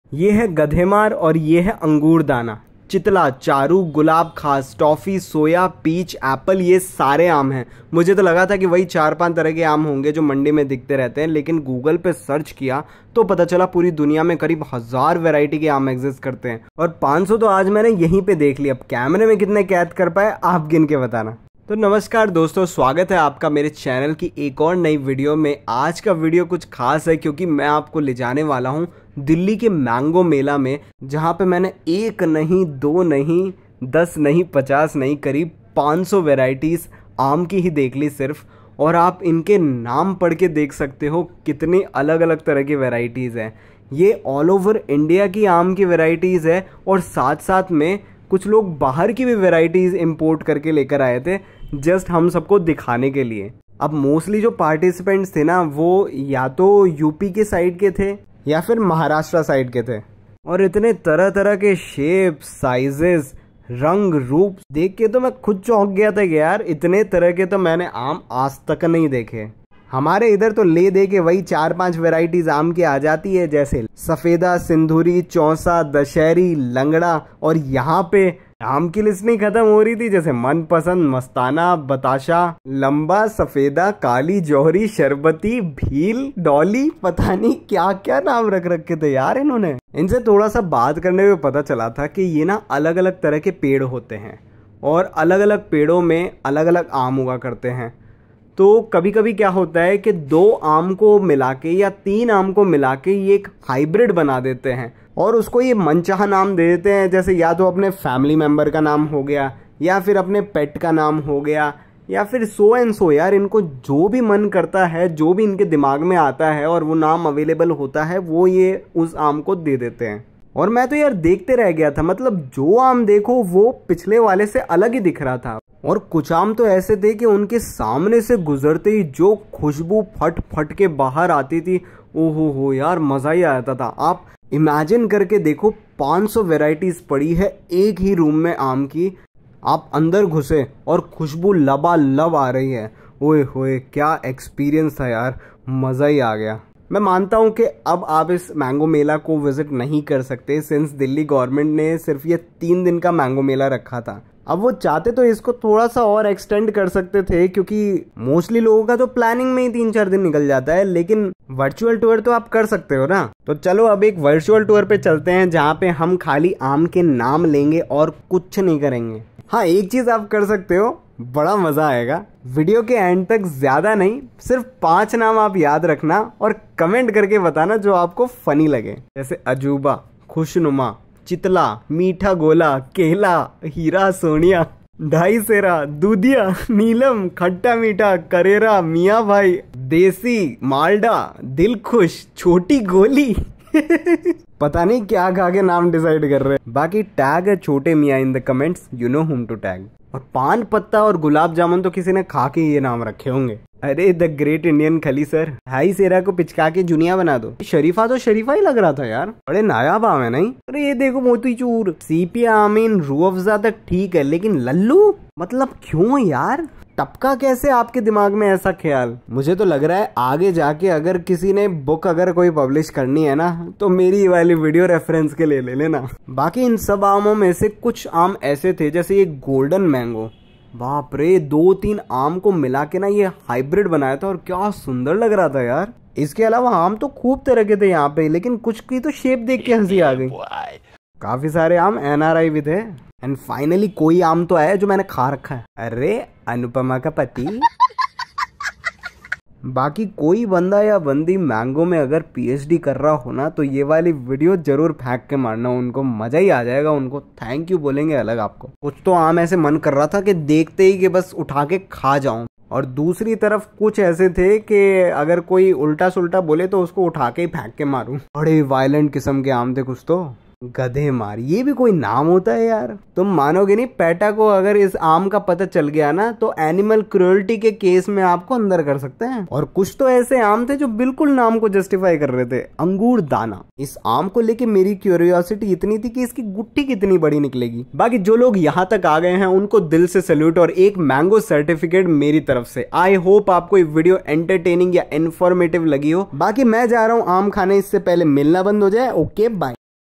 I visited Mango Mela with 500+ Varieties of Mangoes 🥭
Get ready for a sweet and tangy adventure with Backpacker Indian! 🥭 Join me as I explore the vibrant Mango Mela festival in India, showcasing an astonishing 500+ varieties of mangoes! 🌴

In this video, you'll discover:

- The diverse world of Indian mangoes, from Alphonso to Dussehri and beyond!
- The unique flavors, textures, and aromas of each variety
- The cultural significance of mangoes in Indian cuisine and traditions
- Tips and insights from mango farmers and experts

Whether you're a foodie, a fruit enthusiast, or simply curious about Indian culture, this video is a must-watch! So, come and indulge in the sweetest experience of the summer with me! 🌟

Share your thoughts and experiences in the comments.